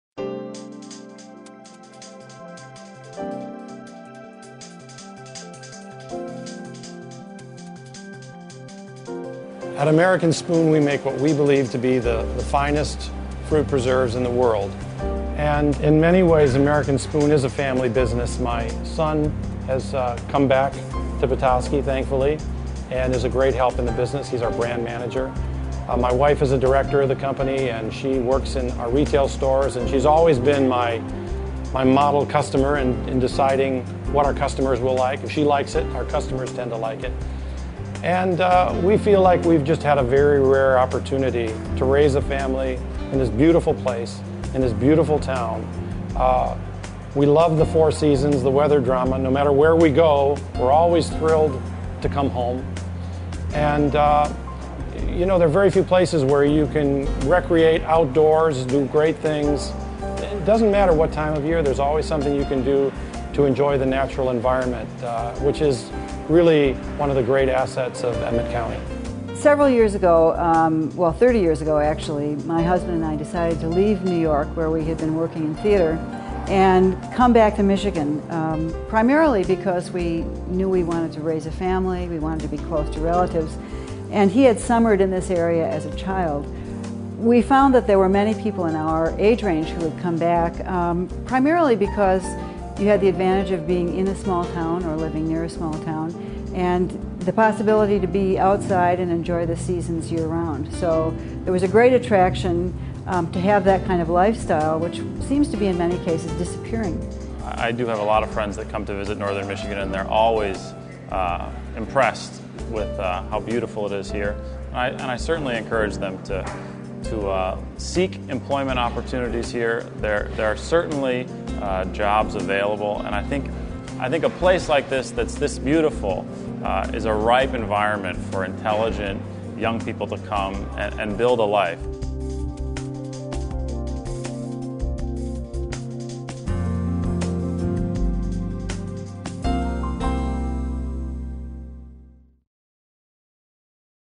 Justin Rashid on Why I Live and Work in Emmet County
Justin Rashid of American Spoon Foods explains why he lives and works in Emmet County, Michigan